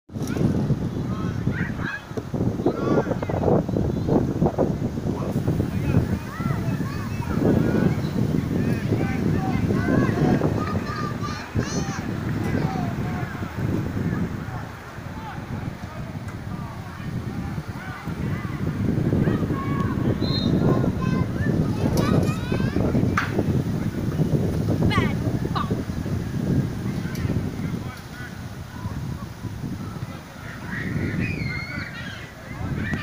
Star Athletic Academy 2 ⚽ 1 BAA
Youth League U11s:
@LenniBizzle_17 and SAA against BAA.
🏆 Youth League
📆 18th January
🏟  Goose Gosling Field